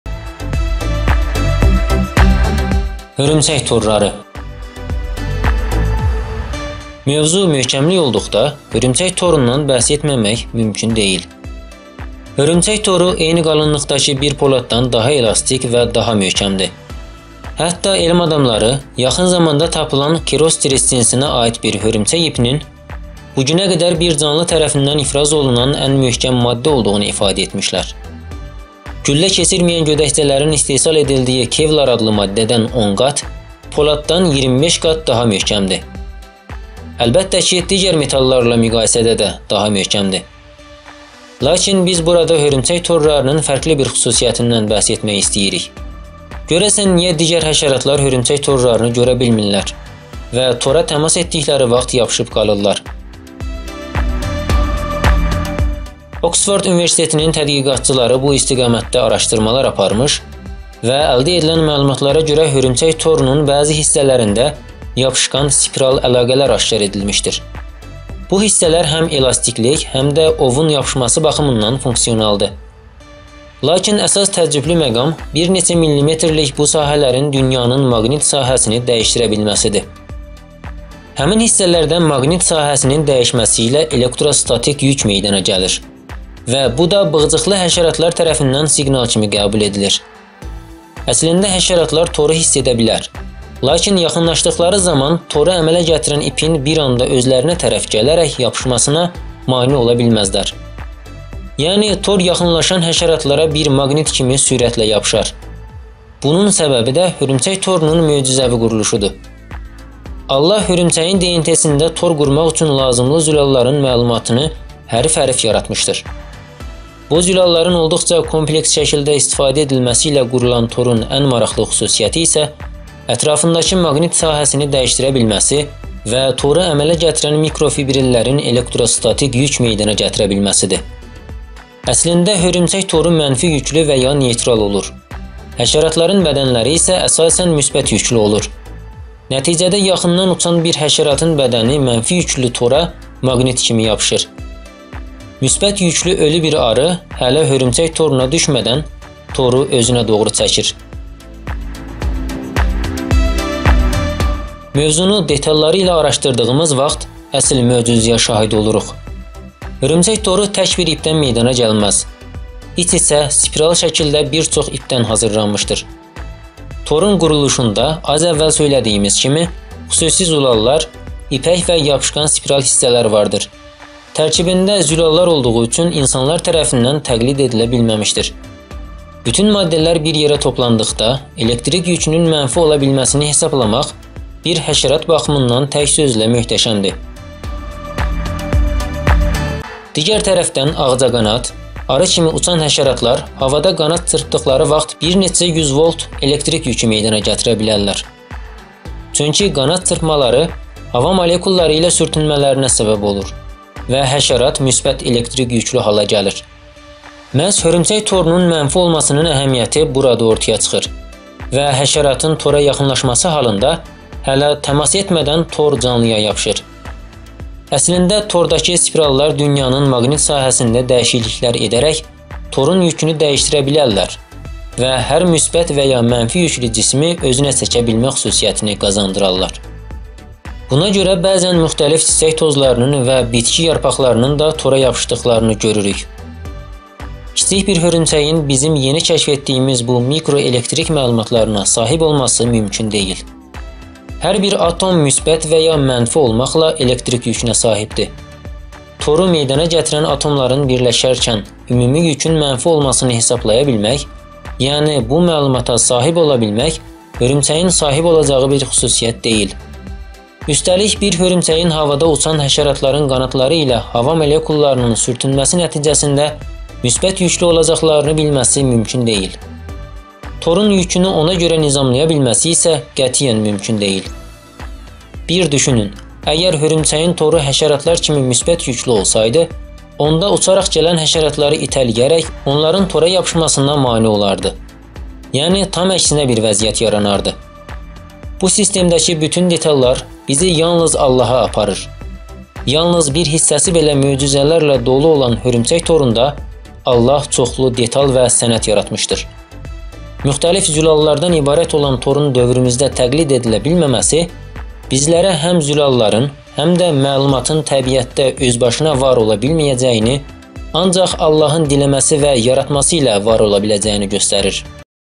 Hörümçək torları
Mövzu möhkəmlik olduqda hörümçək torundan bəhs etməmək mümkün deyil. Hörümçək toru eyni qalınlıqdakı bir poladdan daha elastik və daha möhkəmdir. Hətta elm adamları yaxın zamanda tapılan Caerostris (Kerostris) cinsinə aid bir hörümçək ipinin bu günə qədər bir canlı tərəfindən ifraz olunan ən möhkəm maddə olduğunu ifadə etmişlər. Güllə keçirməyən gödəkcələrin istehsal edildiyi kevlar adlı maddədən 10 qat, poladdan 25 qat daha möhkəmdir. Əlbəttə ki, digər metallarla müqayisədə də daha möhkəmdir.

Lakin biz burada hörümçək torlarının fərqli bir xüsusiyyətindən bəhs etmək istəyirik. Görəsən niyə digər həşəratlar hörümçək torlarını görə bilmirlər və Niyə həşəratlar tora təmas etdikləri vaxt yapışıb qalırlar?

Oksford Universitetinin tədqiqatçıları bu istiqamətdə araşdırmalar aparmış və əldə edilən məlumatlara görə hörümçək torunun bəzi hissələrində yapışqan spiral əlaqələr aşkar edilmişdir. Bu hissələr həm elastiklik, həm də ovun yapışması baxımından funksionaldır, lakin əsas təəccüblü məqam, bir neçə millimetrlik bu sahələrin dünyanın maqnit sahəsini dəyişdirə bilməsidir.

Həmin hissələrdə maqnit sahəsinin dəyişməsi ilə elektrostatik yük meydana gəlir və bu da bığcıqlı həşəratlar tərəfindən siqnal kimi qəbul edilir. Əslində həşəratlar toru hiss edə bilər, lakin yaxınlaşdıqları zaman toru əmələ gətirən ipin bir anda özlərinə tərəf gələrək yapışmasına maneə ola bilməzlər. Yəni tor yaxınlaşan həşəratlara bir maqnit kimi sürətlə yapışar. Bunun səbəbi də hörümçək torunun möcüzəvi quruluşudur. Allah hörümçəyin DNT-sində tor qurmaq üçün lazımlı zülalların məlumatını hərf-hərf yaratmışdır. Bu zülalların olduqca kompleks şəkildə istifadə edilməsi ilə qurulan torun ən maraqlı xüsusiyyəti isə ətrafındakı maqnit sahəsini dəyişdirə bilməsi və toru əmələ gətirən mikrofibrillərin elektrostatik yük meydana gətirə bilməsidir. Əslində hörümçək toru mənfi yüklü və ya neytral olur. Həşəratların bədənləri isə əsasən müsbət yüklü olur. Nəticədə yaxından uçan bir həşəratın bədəni mənfi yüklü tora maqnit kimi yapışır.

Müsbət yüklü ölü bir arı, hələ hörümçək toruna düşmədən, toru özünə doğru çəkir.

Mövzunu detalları ilə araşdırdığımız vaxt əsl möcüzəyə şahid oluruq. Hörümçək toru tək bir ipdən meydana gəlməz, iç-içə, spiral şəkilində bir çox ipdən hazırlanmışdır. Torun quruluşunda az əvvəl söylədiyimiz kimi xüsusi zülallar, ipək və yapışqan spiral hissələr vardır. Tərkibində zülallar olduğu üçün insanlar tərəfindən təqlid edilə bilməmişdir. Bütün maddələr bir yerə toplandıqda elektrik yükünün mənfi ola bilməsini hesablamaq bir həşərat baxımından tək sözlə möhtəşəmdir. Digər tərəfdən ağcaqanad, arı kimi uçan həşəratlar havada qanad çırpdıqları vaxt bir neçə 100 volt elektrik yükü meydana gətirə bilərlər. Çünki qanad çırpmaları hava molekulları ilə sürtünmələrinə səbəb olur və həşərat müsbət elektrik yüklü hala gəlir. Məhz hörümçək torunun mənfi olmasının əhəmiyyəti burada ortaya çıxır və həşəratın tora yaxınlaşması halında hələ təmas etmədən tor canlıya yapışır. Əslində tordakı spirallar dünyanın maqnit sahəsində dəyişikliklər edərək torun yükünü dəyişdirə bilərlər və hər müsbət və ya mənfi yüklü cismi özünə çəkə bilmə xüsusiyyətini qazandırarlar. Buna görə, bəzən müxtəlif çiçək tozlarının və bitki yarpaqlarının da tora yapışdığını görərik. Kiçik bir hörümçəyin bizim yeni kəşf etdiyimiz bu mikro elektrik məlumatlarına sahib olması mümkün deyil.

Hər bir atom müsbət və ya mənfi olmaqla elektrik yükünə sahibdir. Toru meydana gətirən atomların birləşərkən ümumi yükün mənfi olmasını hesablaya bilmək, yəni bu məlumata sahib ola bilmək hörümçəyin sahib olacağı bir xüsusiyyət deyil. Üstəlik, bir hörümçəyin havada uçan həşəratların qanadları ilə hava molekullarının sürtünməsi nəticəsində müsbət yüklü olacaqlarını bilməsi mümkün deyil. Torun yükünü ona görə nizamlaya bilməsi isə qətiyyən mümkün deyil. Bir düşünün, əgər hörümçəyin toru həşəratlar kimi müsbət yüklü olsaydı onda uçaraq gələn həşəratları itələyərək onların tora yapışmasına mane olardı, yəni tam əksinə bir vəziyyət yaranardı. Bu sistemdəki bütün detallar bizi yalnız Allaha aparır, yalnız bir hissəsi belə möcüzələrlə dolu olan hörümçək torunda Allah çoxlu detal və sənət yaratmışdır. Müxtəlif zülallardan ibarət olan torun dövrümüzdə təqlid edilə bilməməsi bizlərə həm zülalların, həm də məlumatın təbiətdə özbaşına var ola bilməyəcəyini ancaq Allahın diləməsi və yaratması ilə var ola biləcəyini göstərir.